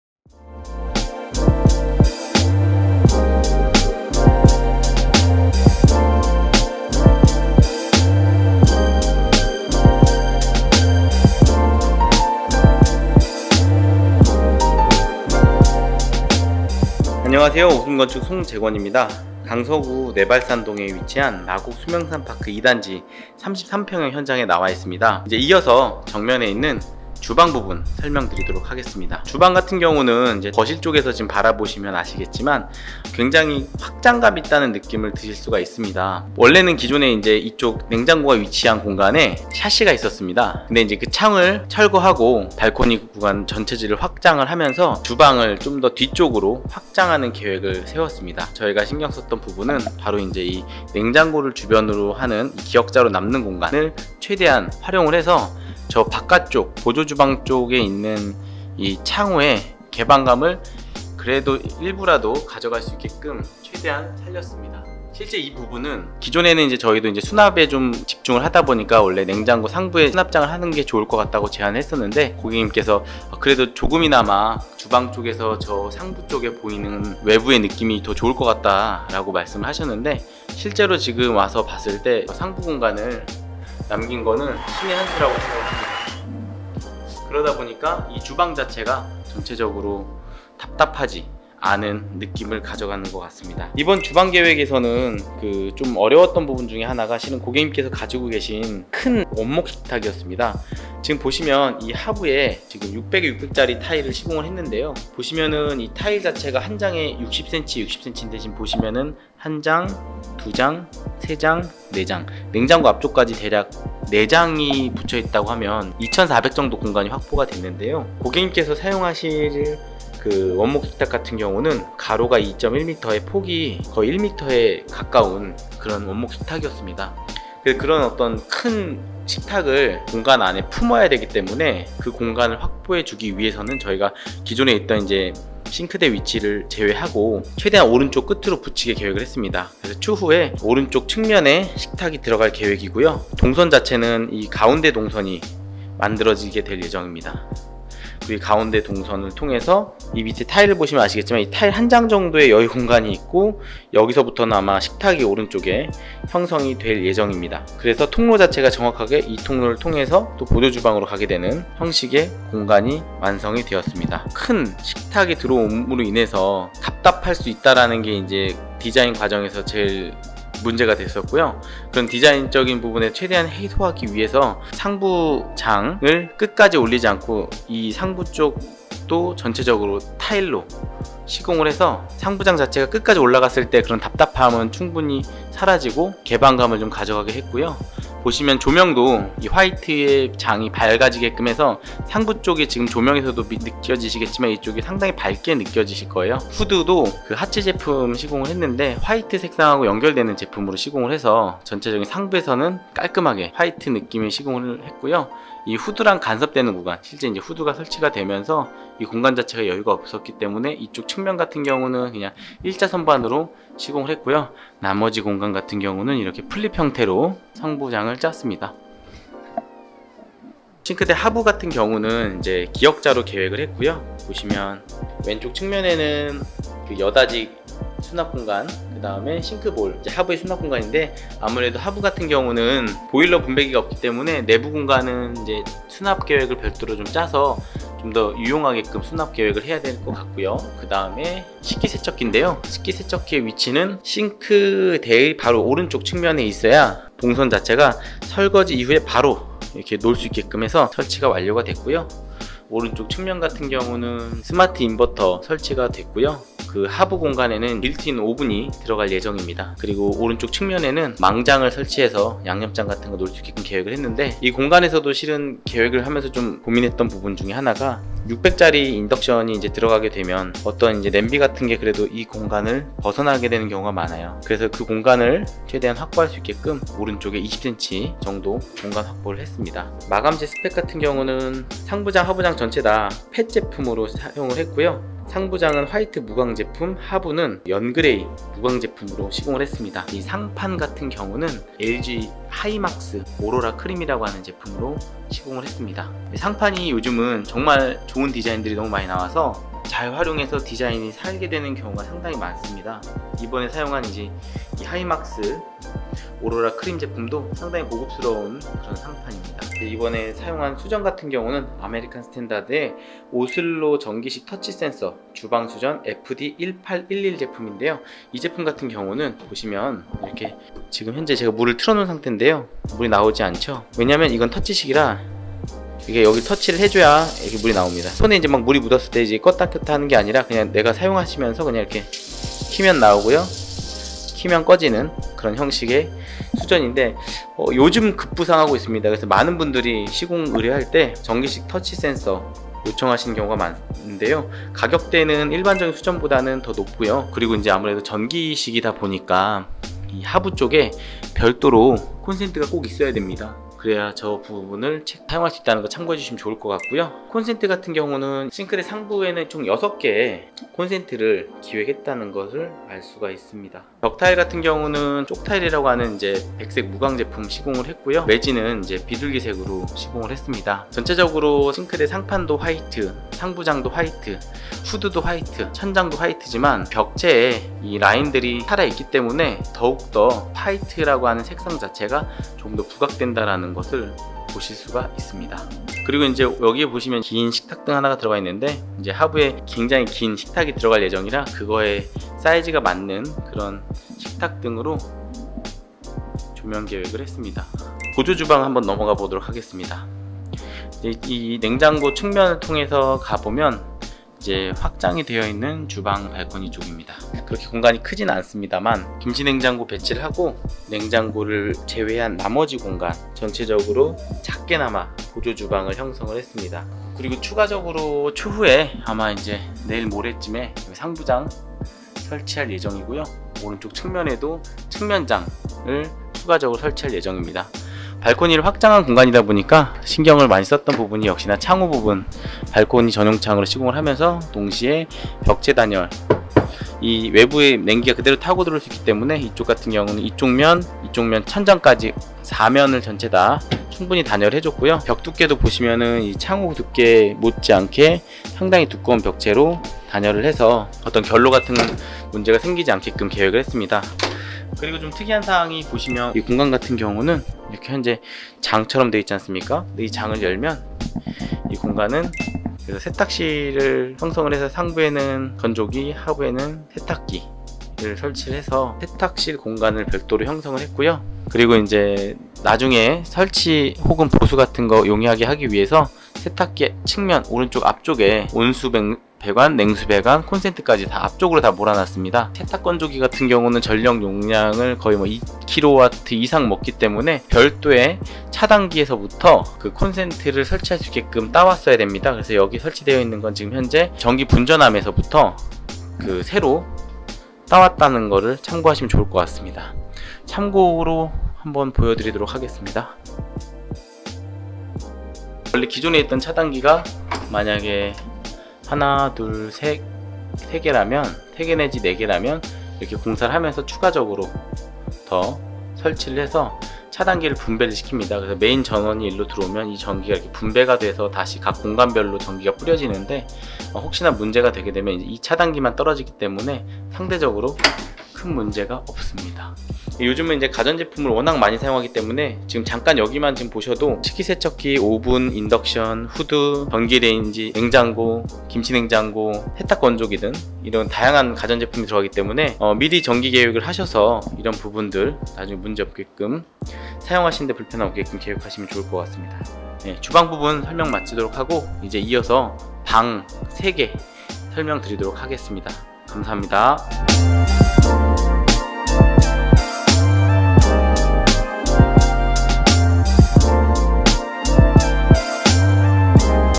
넓은 주방을 위한 획기적인 인테리어가 있다?! ㅣ구조 변경 인테리어ㅣ주방 레이아웃ㅣ33평 아파트 : 5편
마곡 수명산파크 현장으로 인사드려봅니다^^
이번 현장의 주방 인테리어를 공개합니다.
기존의 창호를 철거하여 대형 가전 제품의 배치를 변경하고 가구 구성을 새롭게 계획하여 깔끔하고 더욱 확장된 공간으로 좁은 주방을 넓게 사용할 수 있는 인테리어 대방출 영상 함께 보시죠!

지금부터 마곡 수명산파크현장 5편 확 달라진 주방인테리어편 함께 보실까요!!!

[ 넓은 주방을 위한 획기적인 인테리어가 있다?! ㅣ구조변경 인테리어ㅣ주방 레이아웃ㅣ33평 아파트 : 5편 ]

 00:00  하이라이트 영상
 00:29  주방 발코니 확장
 01:34  대형 원목식탁 계획
 02:52  수납장 디자인(컬러, 가전제품)
 04:40  마감재 소개(수납장, 상판)
 05:10  오슬로 터치센서 수전
 05:57  콘센트&타일 계획
 06:26  식탁등 설치
 06:38  보조주방 형성
 07:37  세탁실 형성
 08:04  전력 계획
 09:08  영상 마무리

- Since 2013 ~